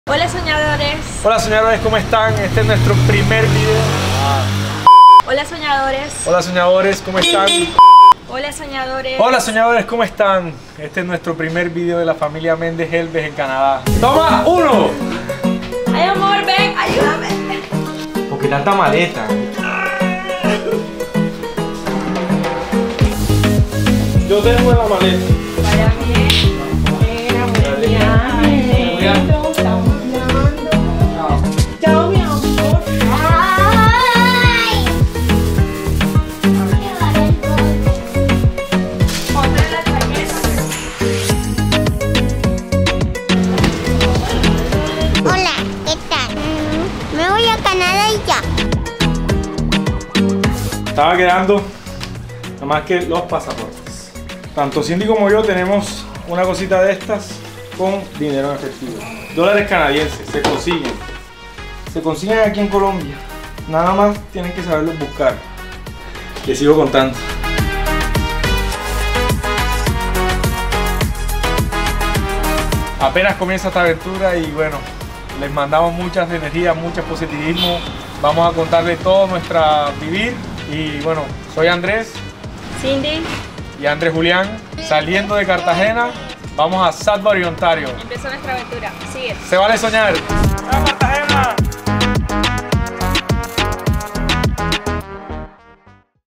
Despedida de Cartagena, SALIMOS PARA CANADA!!!!
Recogemos maletas, nos despedimos de familiares y partimos hacia Barranquilla de Cartagena, para poder viajar a Bogota y de ahi esperamos salir al dia siguiente a Toronto.

Se Vale Soñar!!!